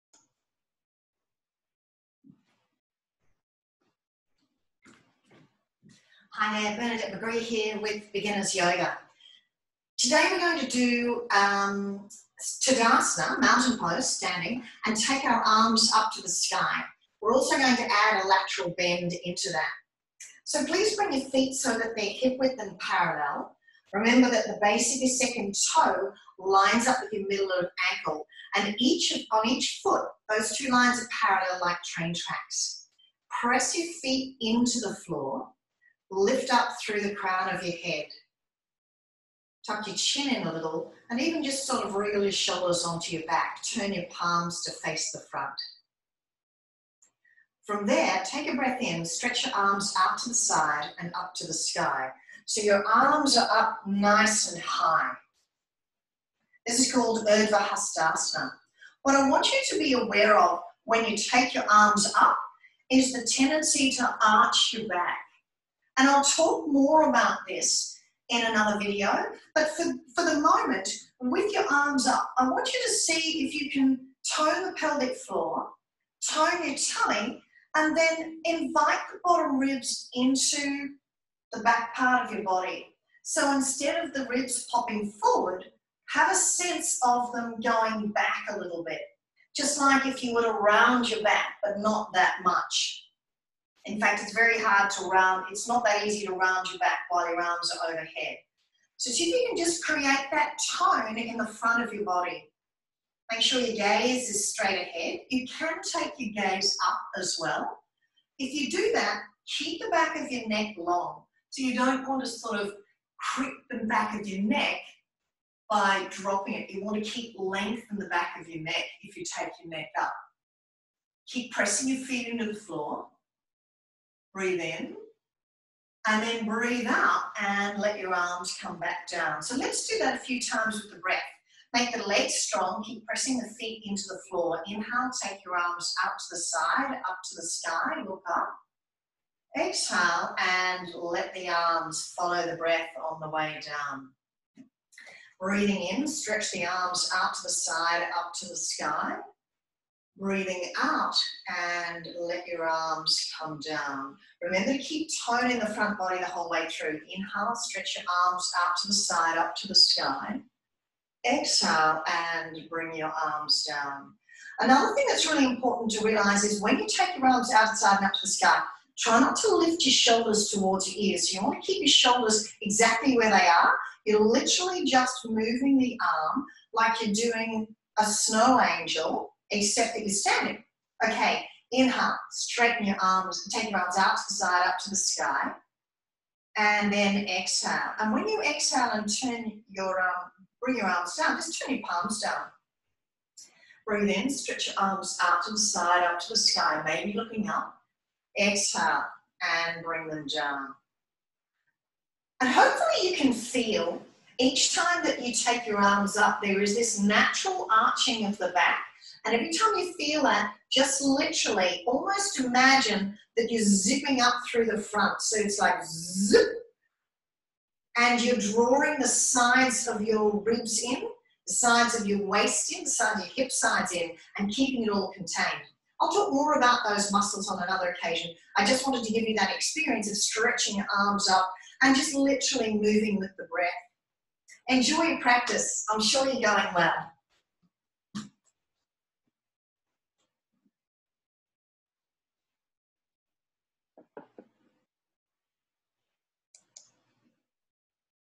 Beginners Yoga Stretch up in Tadasana
Learn how to reach for the stars as you stand tall and take your arms skyward.
You'll begin with Tadasana set up and lengthen from there.